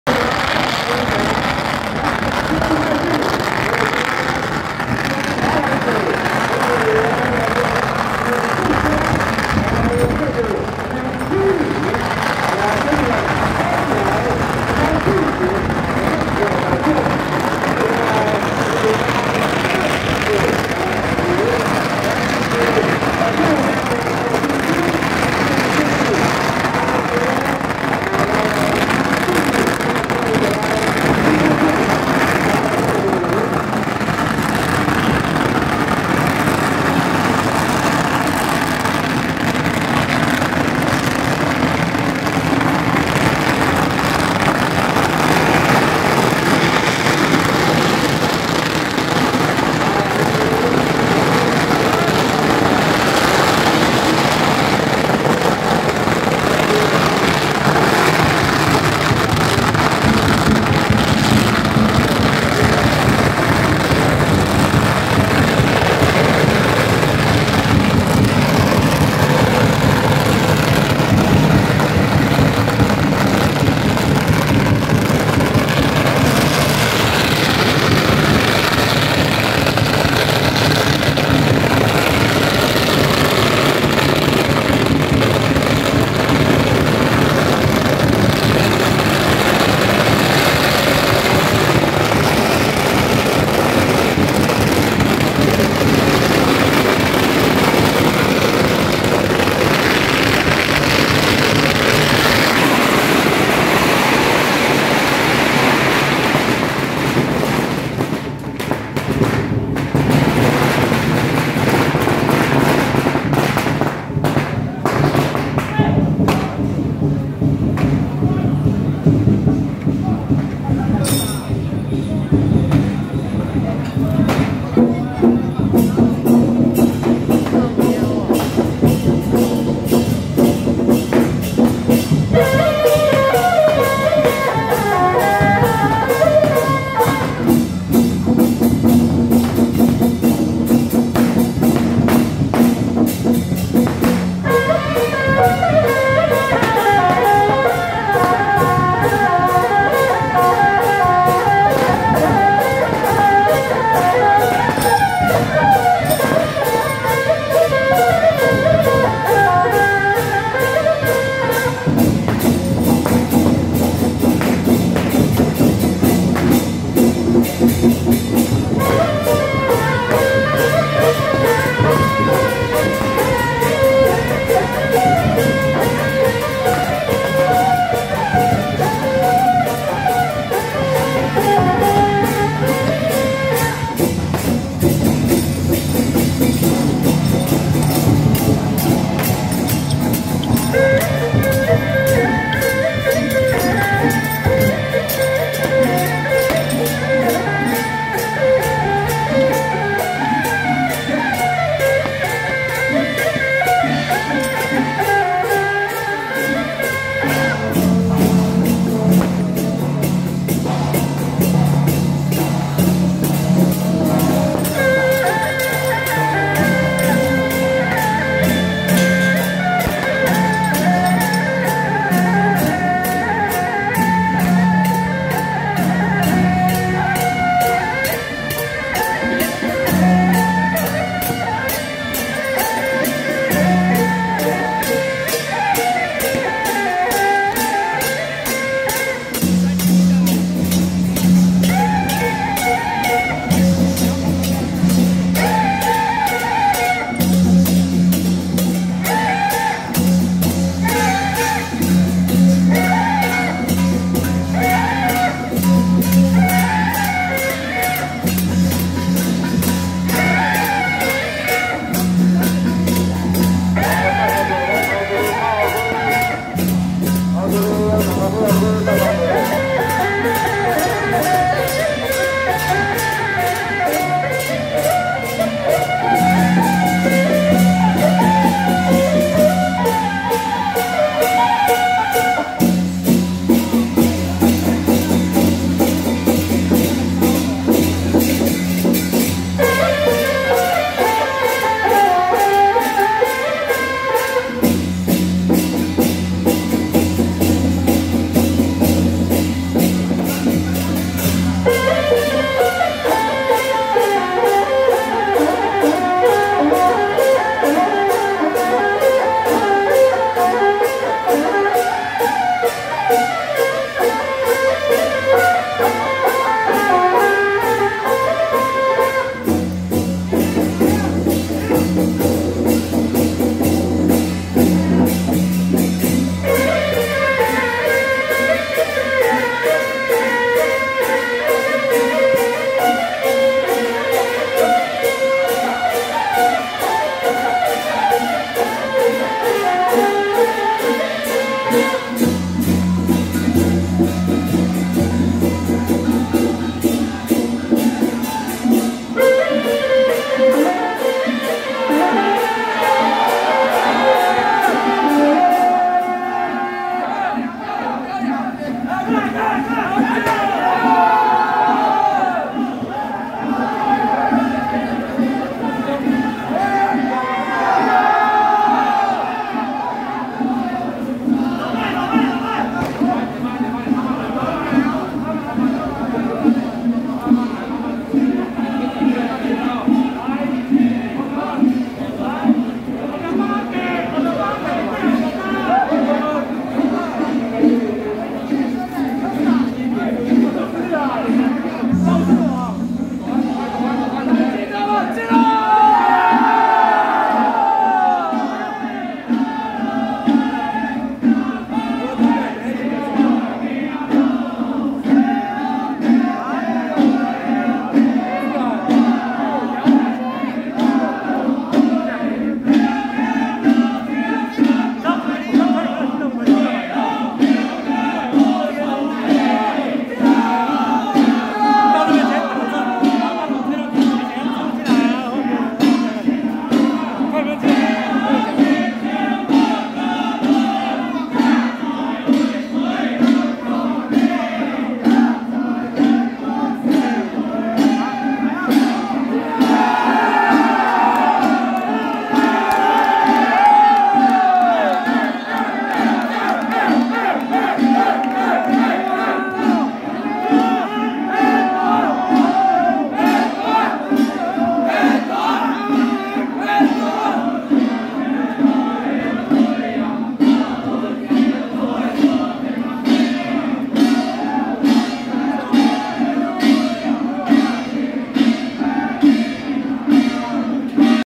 【桃園玄壇-沙淘太子 聖駕(桃聯廣澤會館轎班團)】~110桃園玄壇沙淘太子前往桃園蘆竹誠聖宮建醮大典
桃園玄壇-沙淘太子 聖駕(桃聯廣澤會館轎班團)